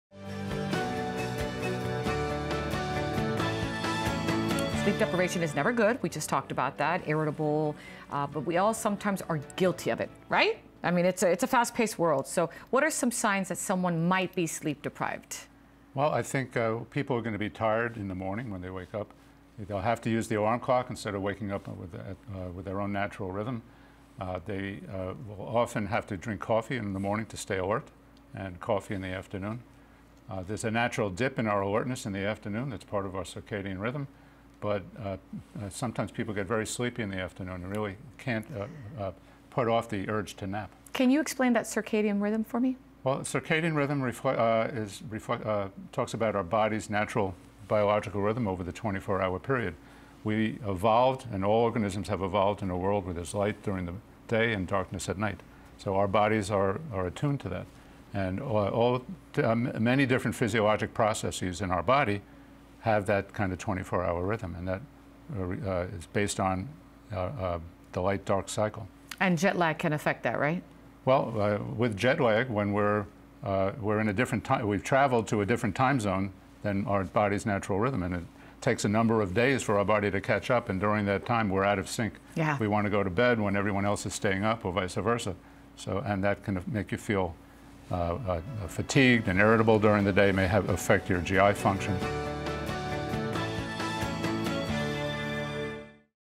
Signs of Sleep Deprivation
Dr. Jeremy Tabak, Medical Director of the Galloway Sleep Center with Baptist Health South Florida, explains when people might be sleep-deprived they’re tired in the morning when they wake up, they’ll have to use the alarm clock instead of waking up with their own natural rhythm and sometimes they get very sleepy in the afternoon, among others signs of sleep deprivation.
Dr. Tabak describes circadian rhythm is about natural biological rhythm over the 24-hour period and it is based on the light-dark cycle.
Jet lag also can affect the quality of sleep, he says.